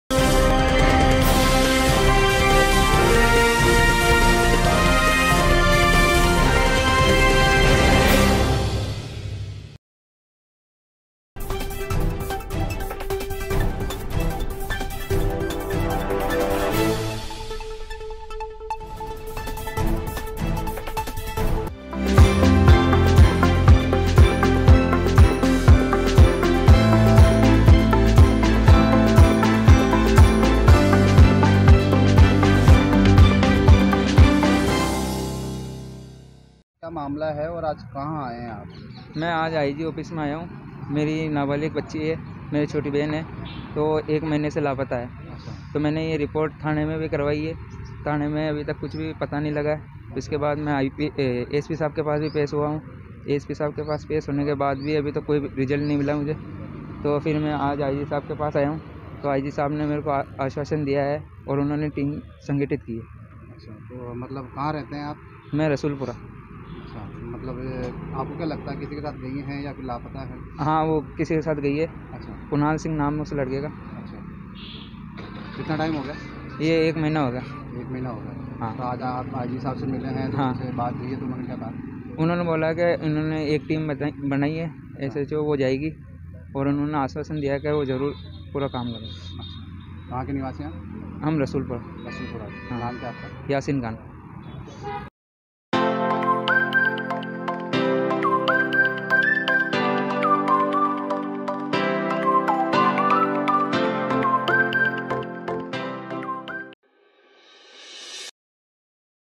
पीड़ित परिवार ने आईजी से लगाई न्याय की गुहार 1 महीने से नाबालिक लड़की के लापता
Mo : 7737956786